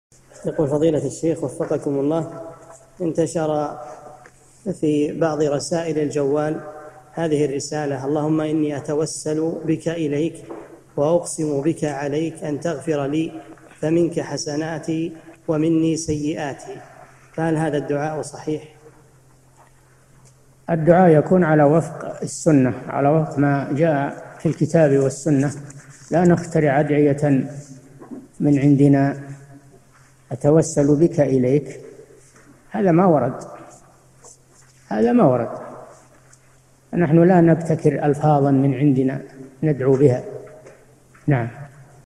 [976 /1350] انتشر في رسائل الجوال هذه الرسالة: "اللهم إني أتوسل بك إليك، وأقسم بك عليك أن تغفر لي،..
-[976 /1350] السؤال: انتشر في بعض رسائل الجوال هذه الرسالة: " اللهم إني أتوسل بك إليك، وأقسم بك عليك أن تغفر لي، فمنك حسناتي، ومني سيئاتي "، فهل هذا الدعاء صحيح؟
الجواب: الْدُّعَاء يكون عَلَىٰ وفق السنَّة، عَلَىٰ وفق ما جاء في الكتاب وَالسنَّة، لا نخترع أدعيةً من عندنا "أتوسل بك إليك"؟! هٰذَا ما ورد، نحن لا نبتكر ألفاظًا من عندنا ندعو بها.
للاشتراك في قناة الشيخ صالح الفوزان
أسئلة وأجوبة من شرح قاعدة جليلة ( 1350 ) مقطع

للاستفسار والإقتراحات والدعم
رئيس الفريق
لمشروع كبار العلماء